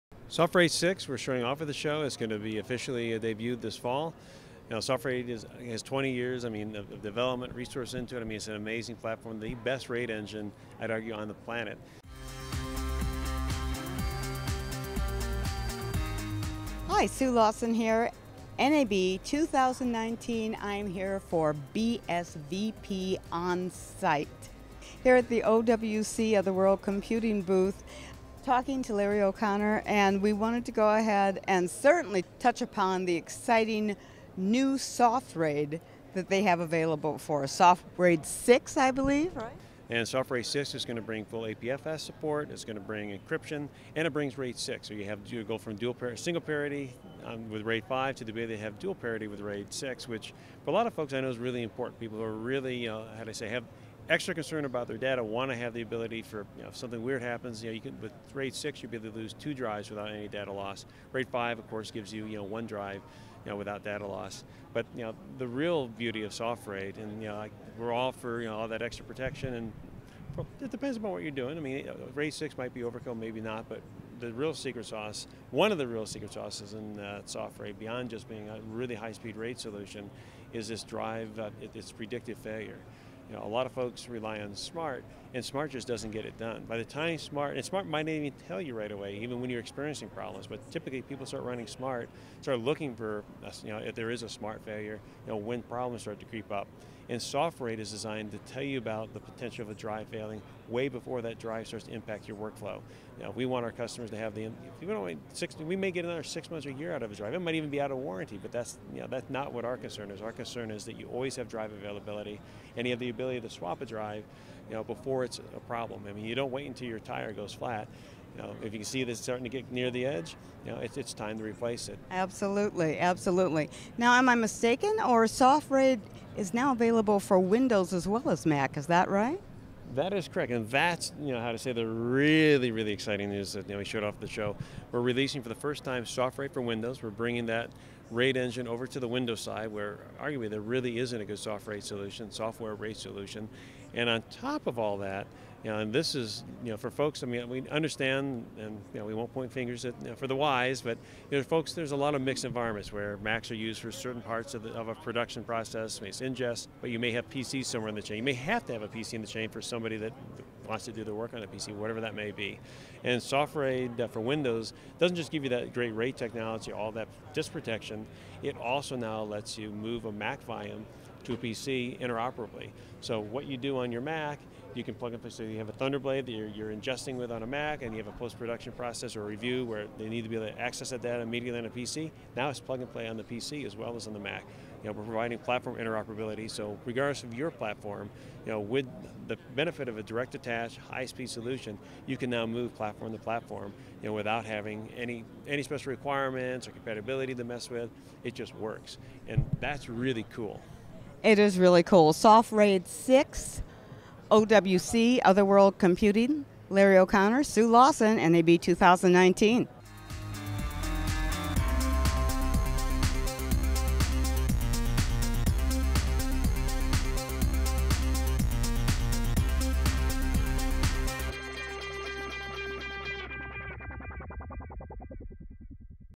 Predictive Hard Drive Failure with OWC SoftRaid
BSVP On-Site: Tech - Predictive Hard Drive Failure with OWC SoftRaid
SoftRAID 6, we're showing off of the show is going to be officially debuted this fall.

SoftRAID is twenty years of development and research into it. It's an amazing platform. The best RAID engine, I'd argue, on the planet.

Hi, Sue Lawson here. NAB 2019. I'm here for BSVP On-Site. Here at the OWC, Other World Computing booth, talking to Larry O'Connor and we wanted to go ahead and certainly touch upon the exciting new SoftRAID that they have available for us. SoftRAID 6, I believe.

SoftRAID is gonna bring fully PFS support. It's gonna bring encryption and it will bring RAID 6 or you can go from single parity with RAID 5 to  dual parity with RAID 6 which but a lot of folks I know is really important. People who have extra concern about their data, want to have the ability for something weird happens you can, but with RAID 6 you really lose two drives without any data loss. RAID 5 of course gives you you know one drive you know without data loss. But the real beauty of SoftRAID and you know we're offering all that extra protection and it's depends on what you're doing. RAID 6 might be overkill maybe not, but the real secret sauce, one of the real secret sauces and software beyond just being a really high speed RAID solution is this drive its predictive failure. A lot of folks rely on S.M.A.R.T. and S.M.A.R.T just doesn't get it done by the tiny S.M.A.R.T. might even tell you right away even when you're experiencing problems. But typically people start running S.M.A.R.T. start looking for problems. If there is a S.M.A.R.T. failure you know when problems start to creep up and SoftRAID is designed to tell you about the potential of a drive failing way before that drive starts to impact your workflow. Now we want our customers to have the information. You may have another six months to a year out of a drive. It might even be out of warranty. But that's not what our concern is. Our concern is that you always have drive availability and you have the ability to swap a drive before it's a problem. You don't wait until your tire goes flat. If you can see it is starting to get near the edge you know it's time to replace it.

Absolutely. Now am I mistaken or is SoftRAID now available for Windows as well as Mac is that right?

That is correct and that's, how you say, the really, really exciting is that we showed off the show we're releasing for the first time SoftRAID for Windows. We're bringing that RAID engine over to the Window side. We're arguing there really isn't a good software RAID solution and on top of all that, you know, this is for folks understand and you know we won't point fingers at for the whys but folks there's a lot of mixed environments where Macs use certain parts of the production process mixed ingest but you may have a PC somewhere in the chain. may have to have a PC in the chain for somebody that wants to do their work on a PC whatever that may be. SoftRAID for Windows doesn't just give you that great rate technology all that disk protection. It also now lets you move a Mac volume to PC inter-operably. So what you do on your Mac you can plug in. You have a ThunderBlade there you're ingesting with on a Mac and you have a post-production process or a review where they need to be able to access at that need it on a PC now is plug and play on the PC as well as on the Mac. We're providing platform interoperability. So regardless of your platform you know with the benefit of a direct-attached high-speed solution you can now move platform to platform without having any special requirements or compatibility to mess with. It just works. That's really cool.

It is really cool. SoftRAID 6. OWC, Other World Computing. Larry O'Connor, Sue Lawson, NAB 2019.